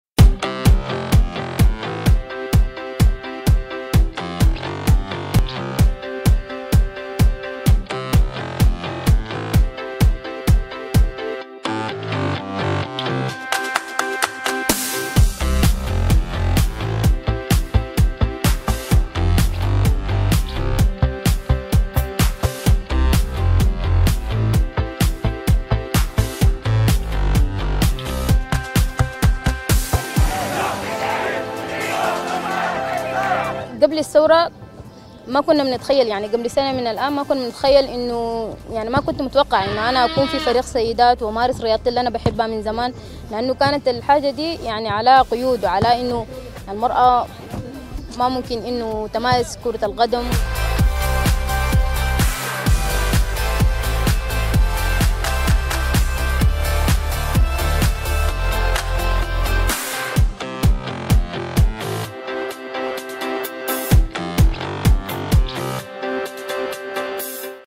Nouvelle révolution au Soudan !💪🏽😍
Nouvelle révolution au Soudan...😍 Les femmes jouent au foot !👩🏽⚽